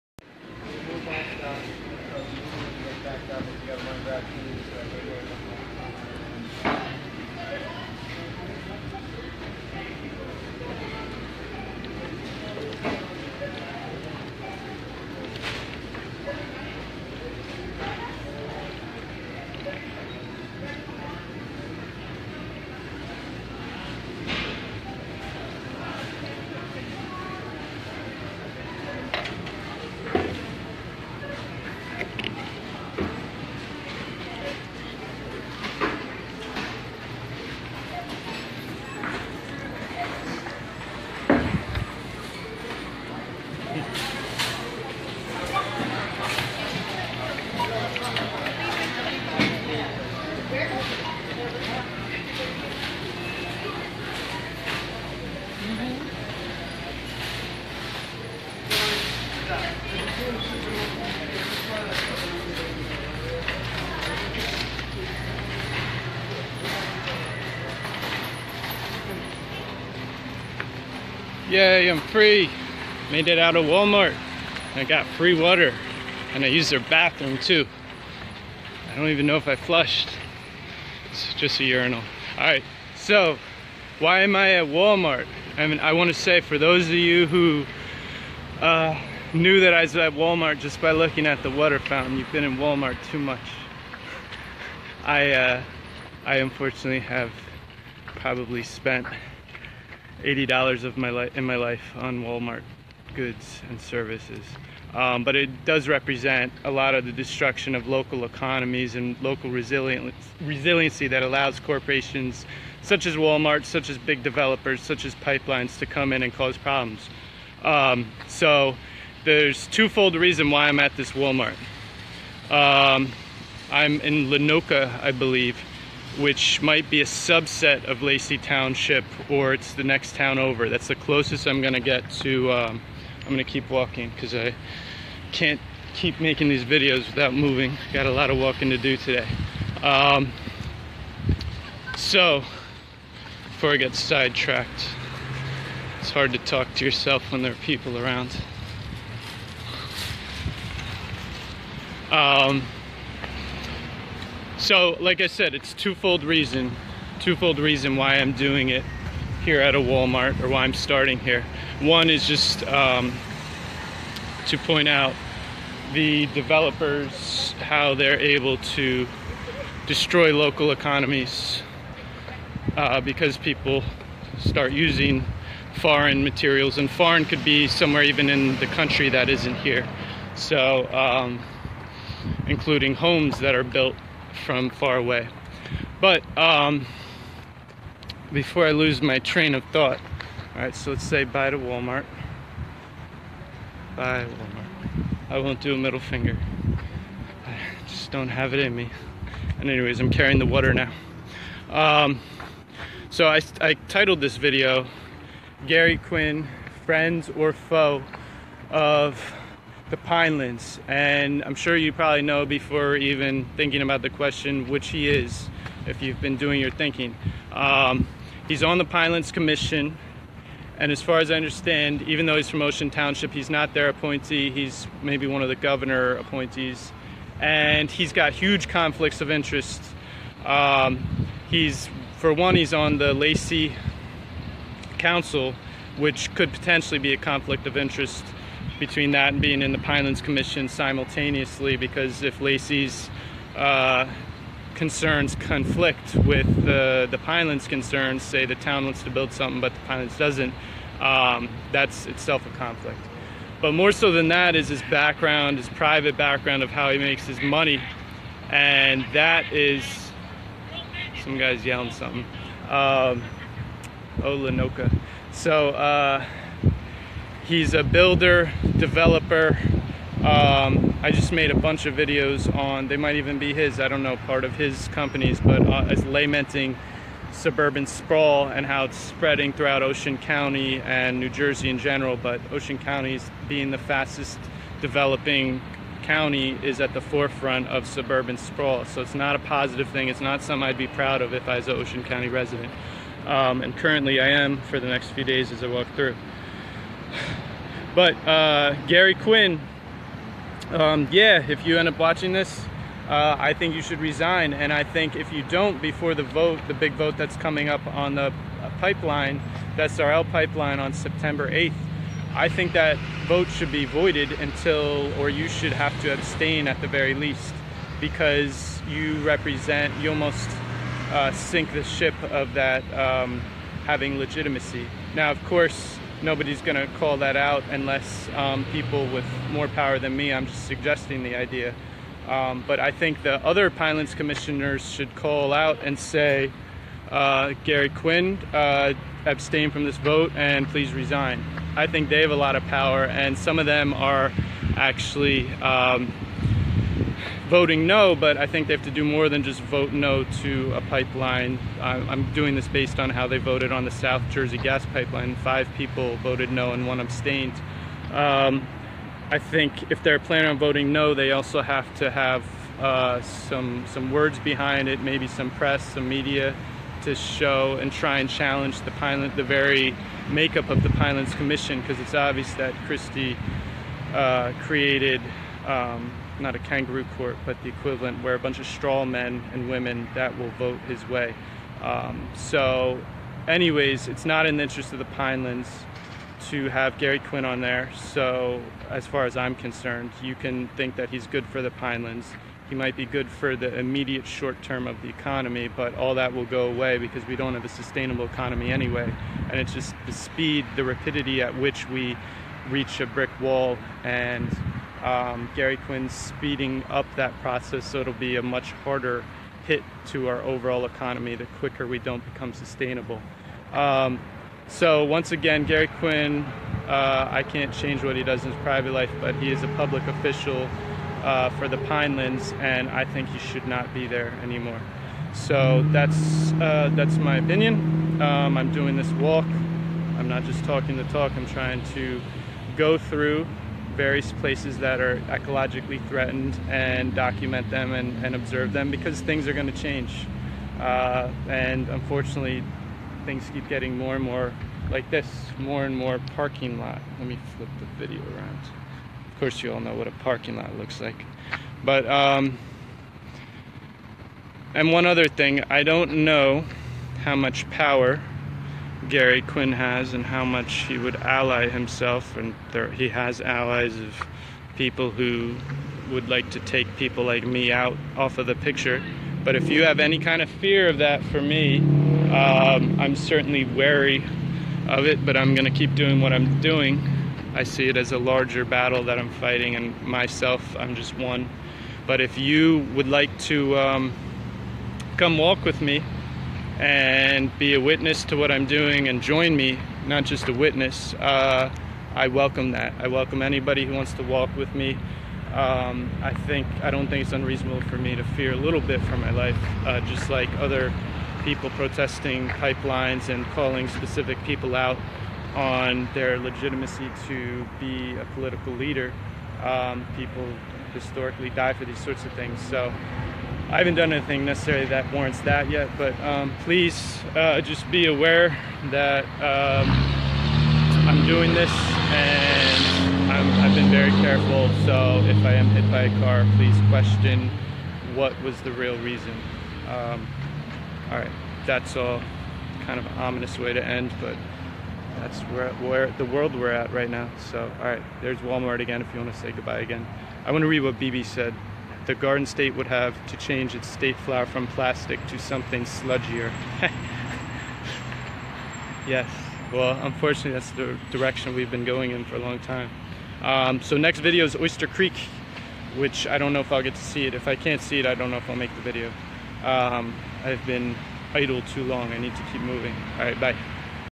Carless Karlos - At Walmart Lanoka Township, Talking About Pinelands Commissioner
This was an Op-Ed on same topic posted later by someone with more details